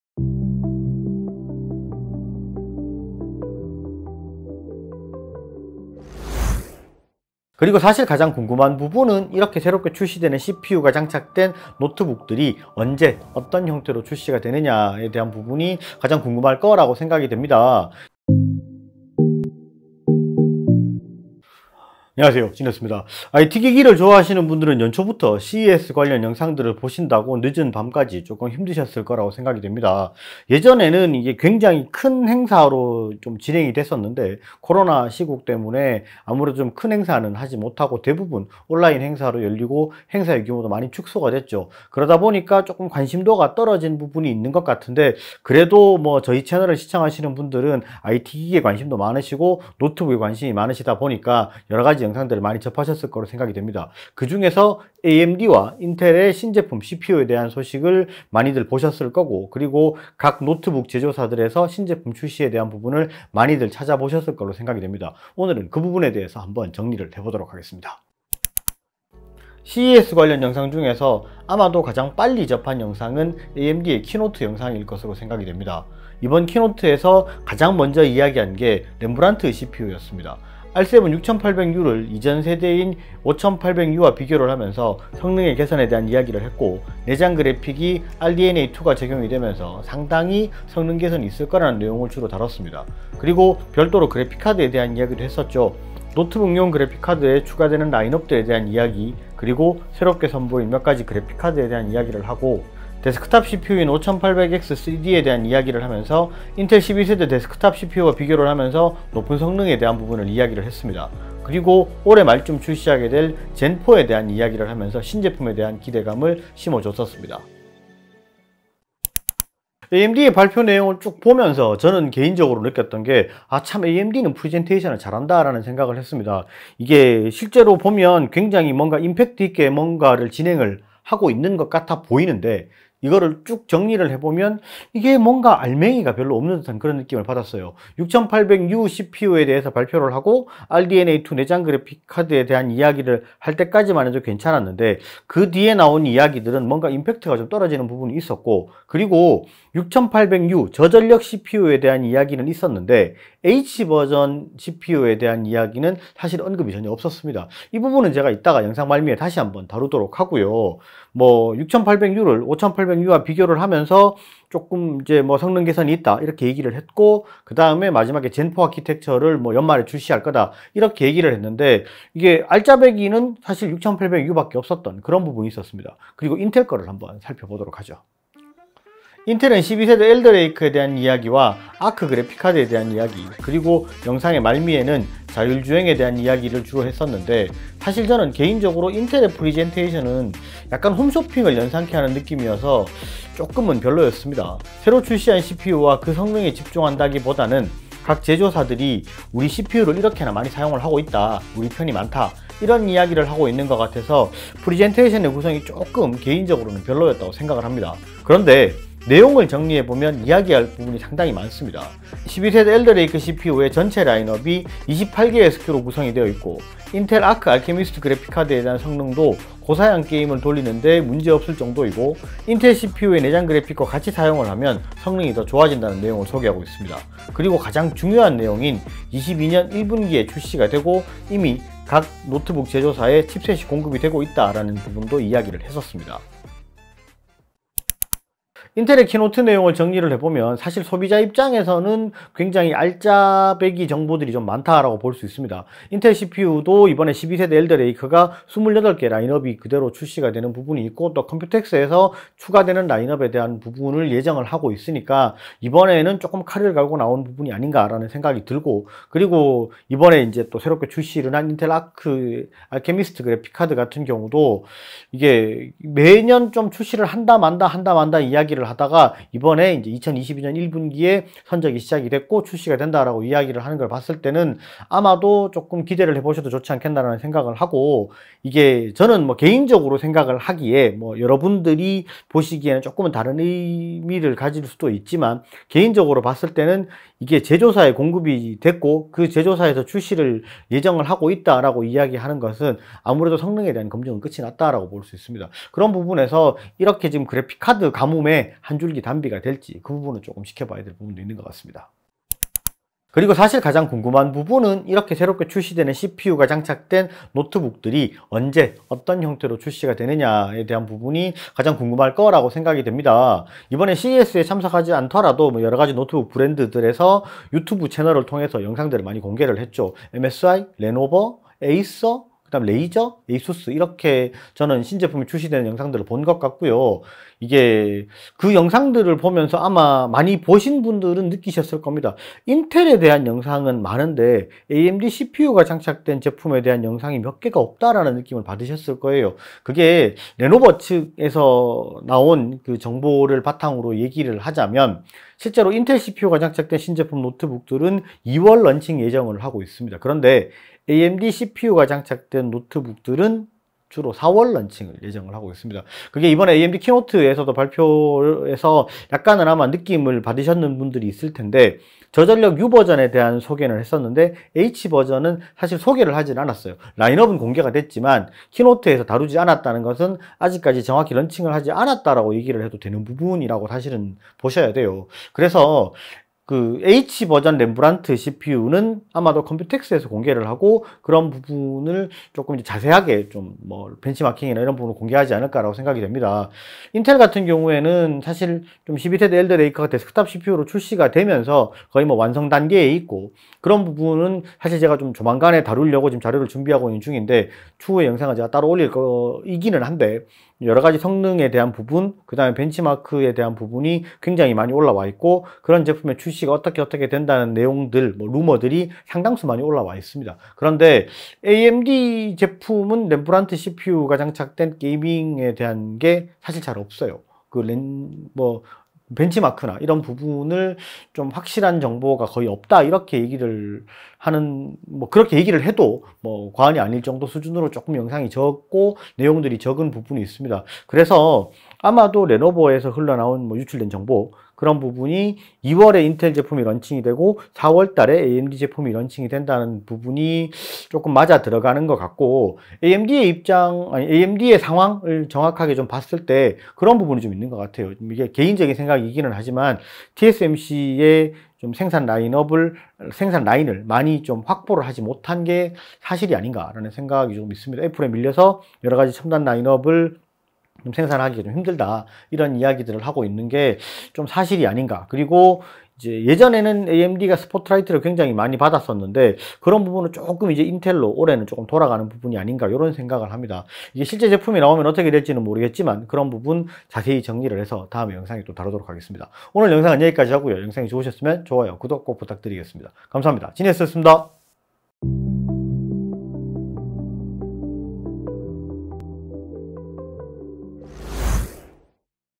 [4K] CES 2022 이후 게이밍 노트북 출시일정은 어떻게 되나요?? [Feat AMD, INTEL 키노트 정리]
CES 2022에서 AMD와 INTEL의 키노트 정리와 이후 게이밍 노트북 출시 일정에 대해서 알아봅시다.